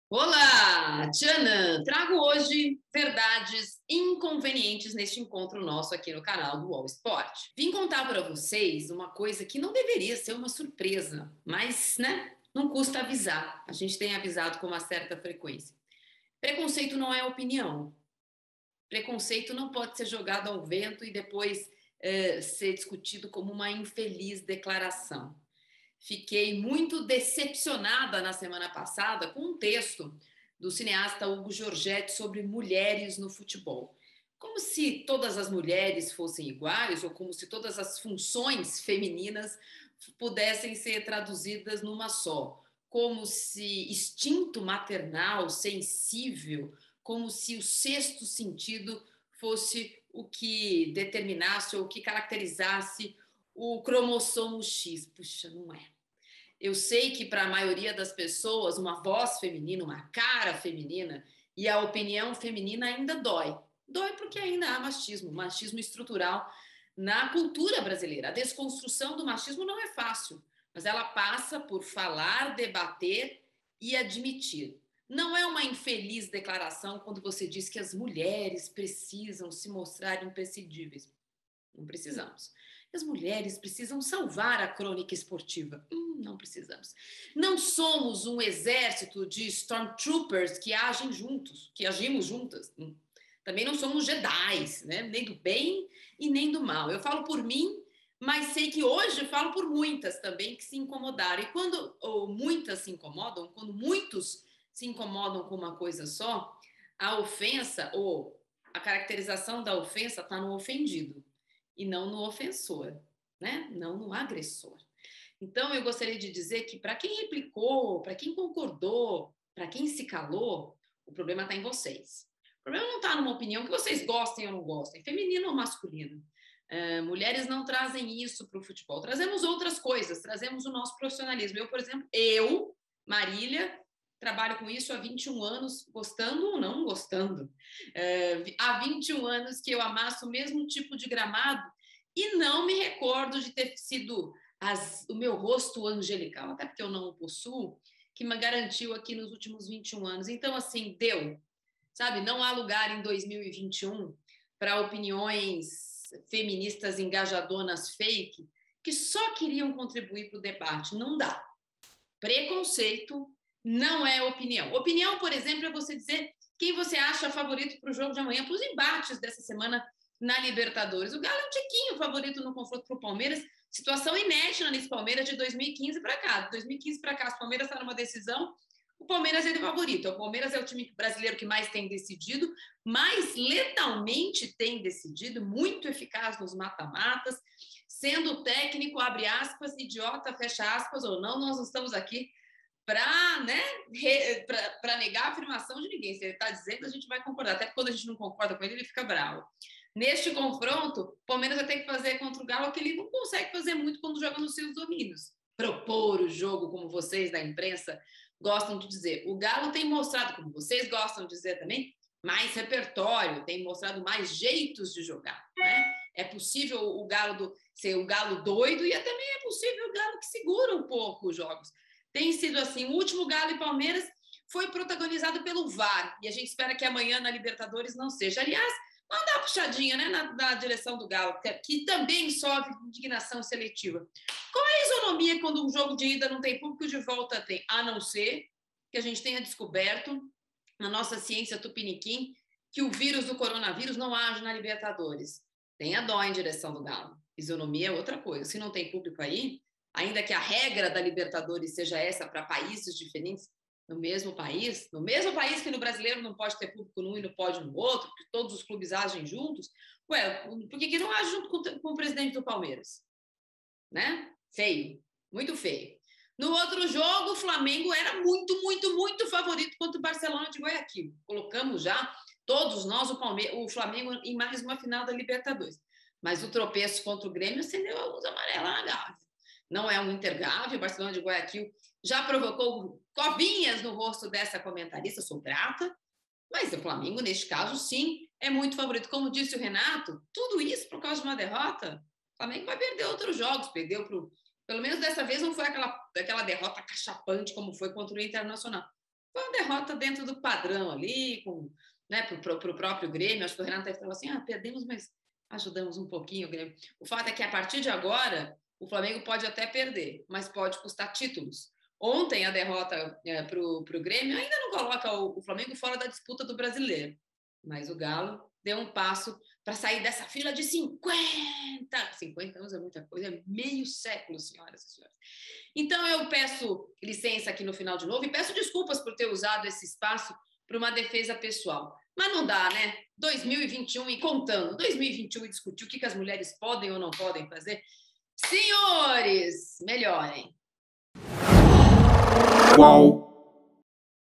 Marília Ruiz: "Não há lugar para opiniões preconceituosas no mundo atual"
A colunista de UOL Esporte Marília Ruiz critica a coluna do cineasta Ugo Giorgetti sobre "Mulheres no Futebol"